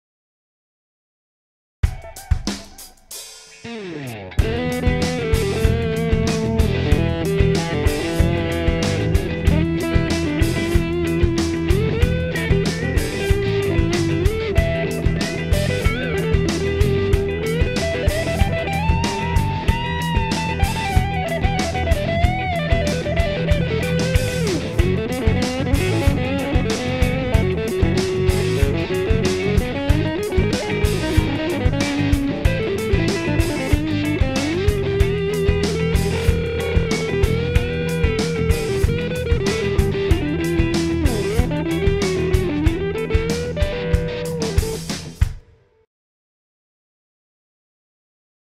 Cakewalk Guitar Tracks Pro 3 - demo
Bass and guitars recorded D.I. and played through the IK Multimedia LE amp sim.
Drum loops from the bundled samples content.
Everything works fine in Windows 10 64 bit.